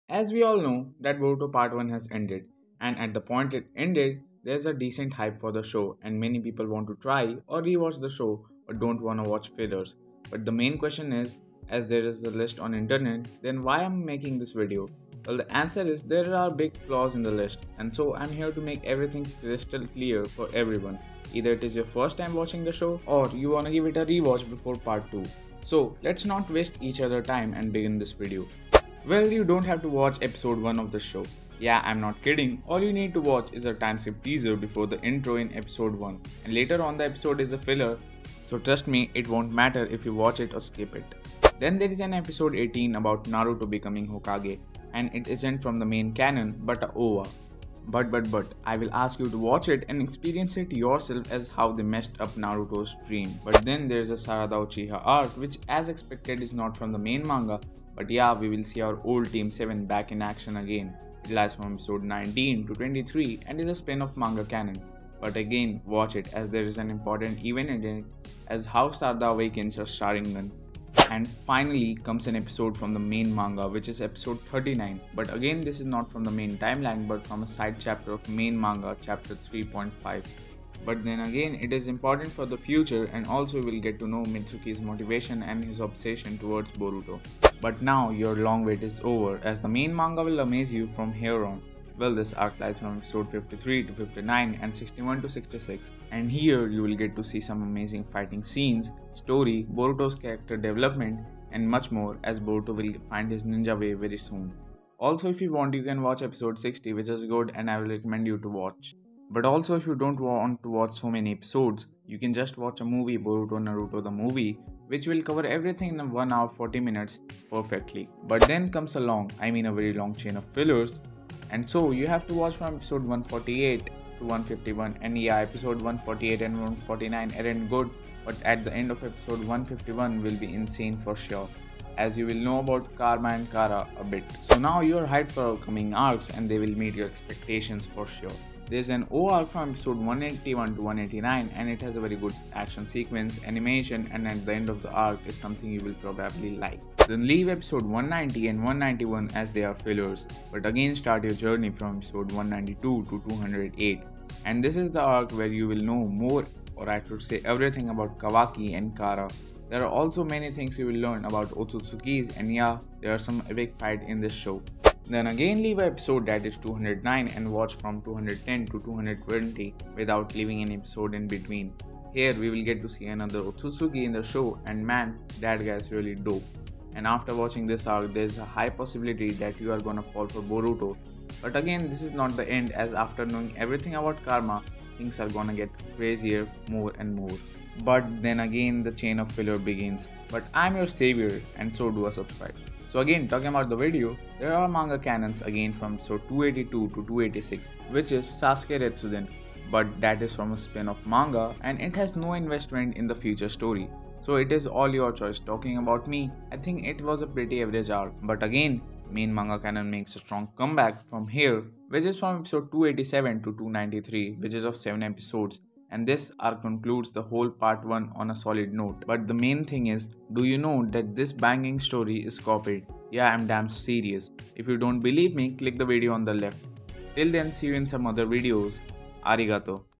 Best Boruto Watch Order in 2023 [Which Fillers To Skip?]
What is the Best Boruto Watch Order After It Has Ended(Latest)?

In this video I will tell you about the right order to watch Boruto After 26 March When Boruto: Naruto Next Generations Part 1 Has OFFICIALLY Ended! If you want to rewatch or start watching boruto then this is the best order to watch Boruto anime as it allows you to end this in fastest way!
Are you a fan of Boruto but tired of sitting through endless filler episodes?
Look no further! In this video, we'll be discussing which Boruto filler episodes you should skip and which ones are actually worth watching.
Check the video for more details!!

Check Out:-
Channel - @senpai_unfiltered
Top 3 Boruto Theories -